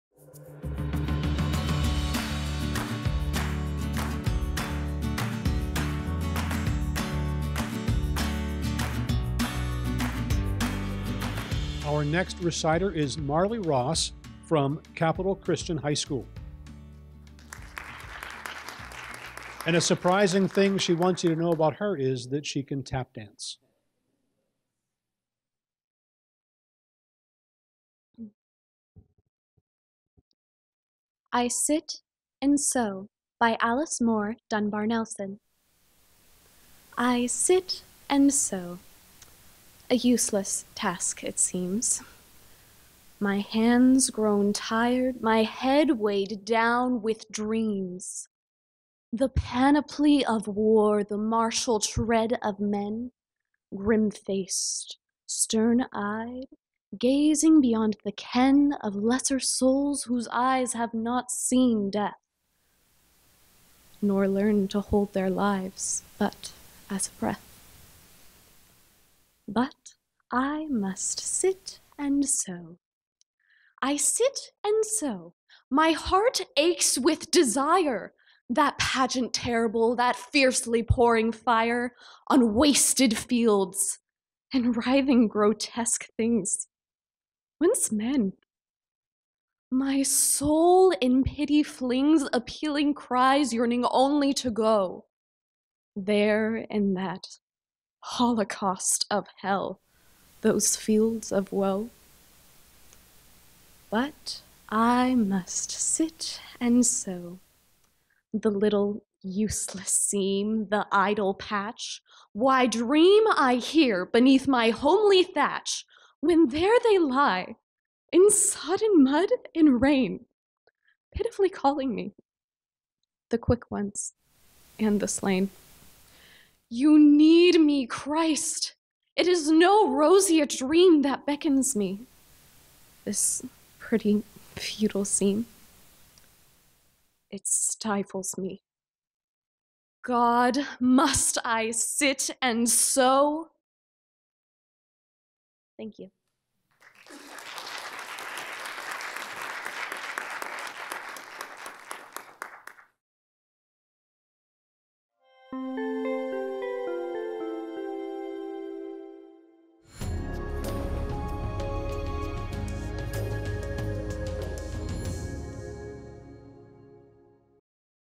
Poetry Out Loud 2018: Marley Ross (Round 2)
Marley Ross from Capital Christian High School performs "I Sit and Sew" by Alice Moore Dunbar-Nelson at Poetry Out Loud 2018.